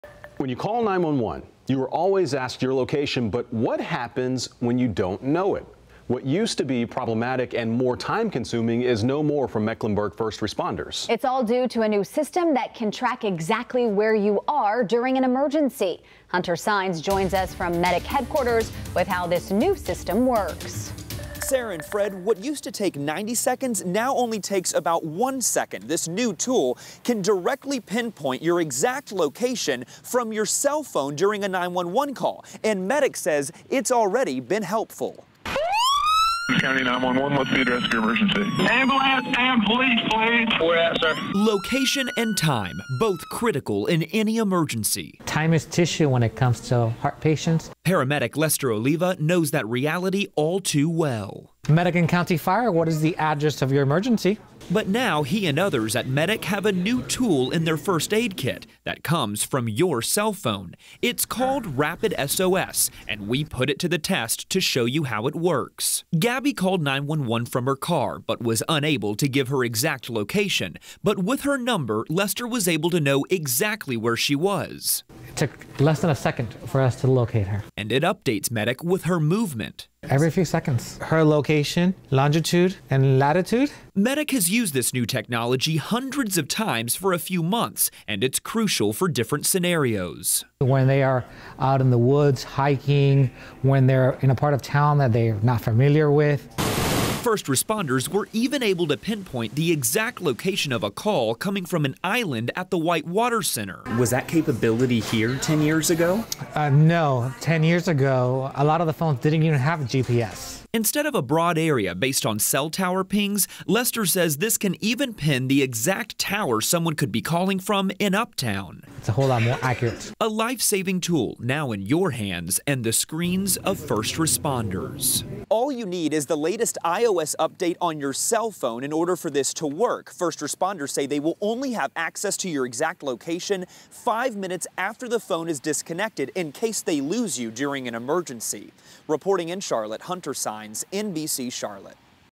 New system helps 911 dispatch and Medic
When you call 911, you're always asked for your location. But what happens when you don't know what that is?